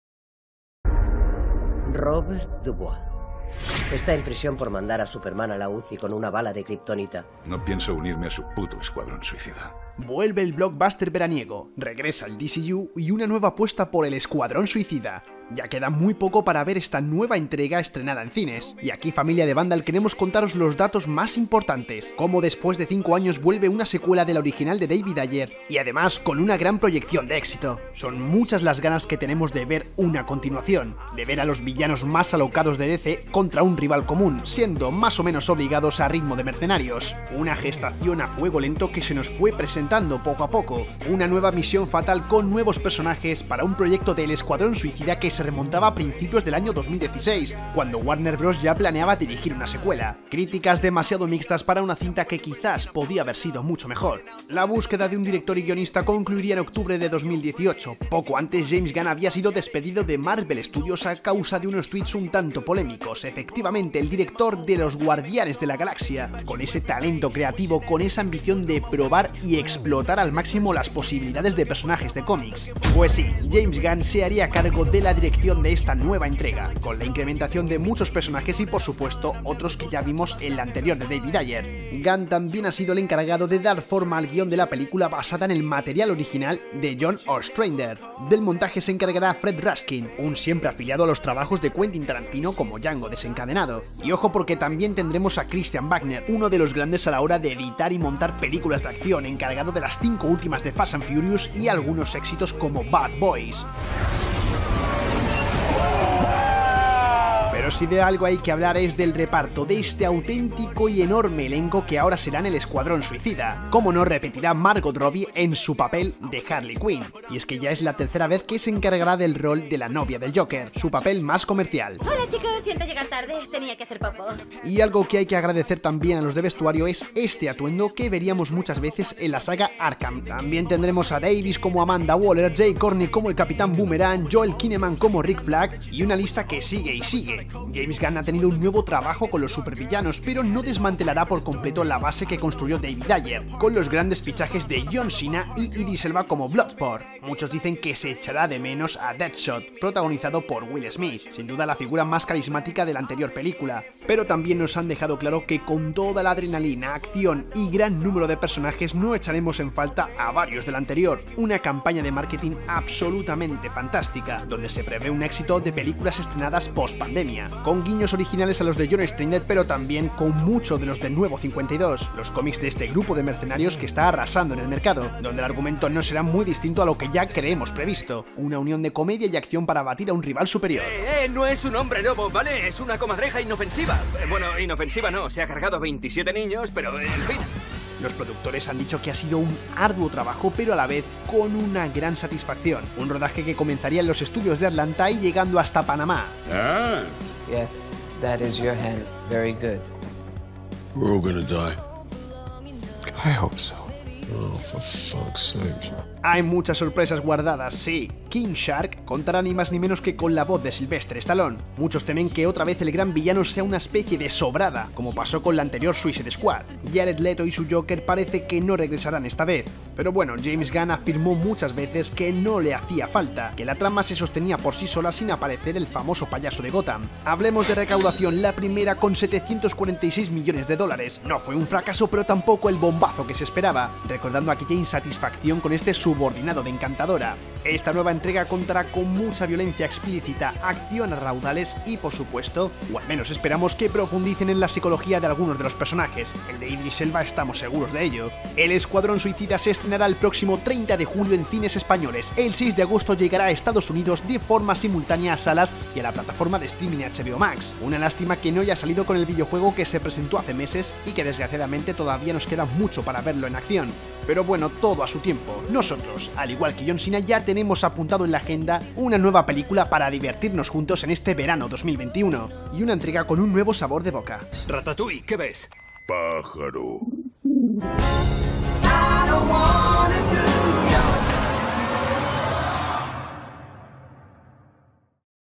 EL ESCUADRÓN SUICIDA: Todo sobre la NUEVA PELÍCULA de JAMES GUNN
Este mes de agosto se estrena en los cines de todo el mundo 'El Escuadrón Suicida', una nueva película protagonizada por un grupo de villanos de DC Comics que en esta ocasión estará dirigida por James Gunn, el director de las películas de 'Guardianes de la Galaxia', y que tiene una pinta estupenda. Te contamos todo lo que sabemos sobre ella.